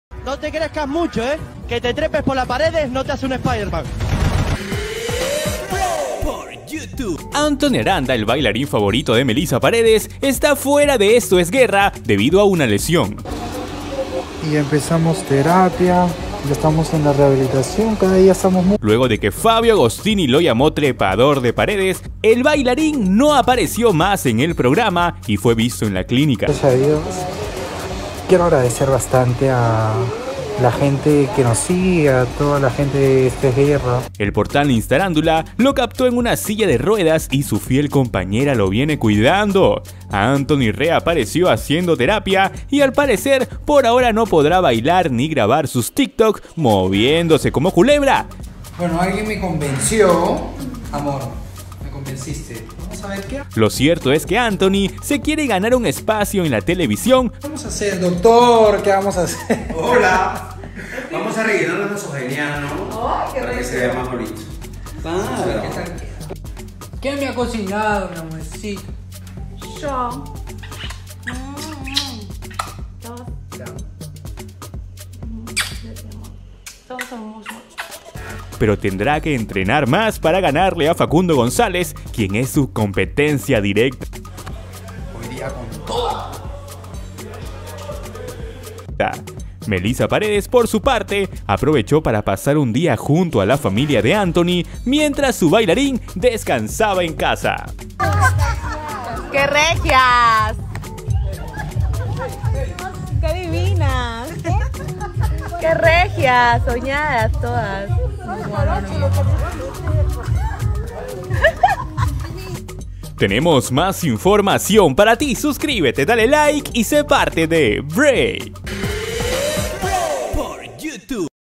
NOVIO DE MELISSA PAREDES YA NO PODRÁ BAILAR NI COMPETIR EN ESTO ES GUERRA?
Anthony Aranda fue visto en silla de ruedas y muletas tras lo sucedido en Esto es guerra.